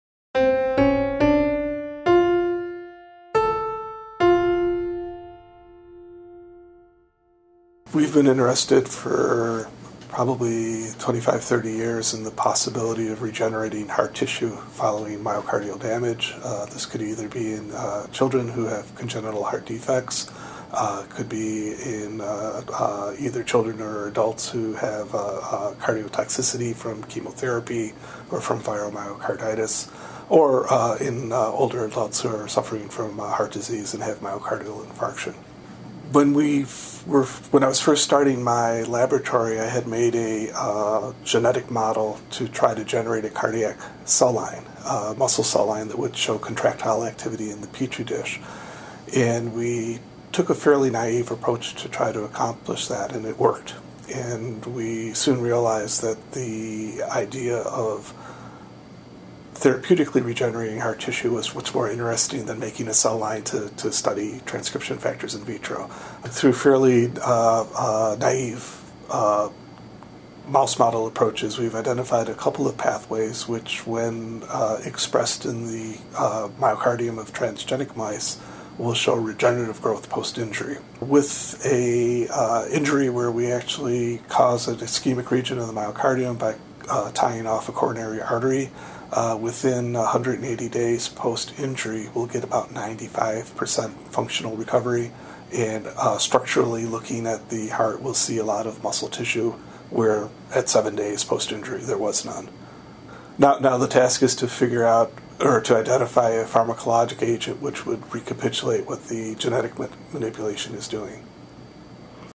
Loren Field on his heart regeneration research
Loren Field, a scientist at Indiana University School of Medicine, talks about his efforts to find ways to repair damaged hearts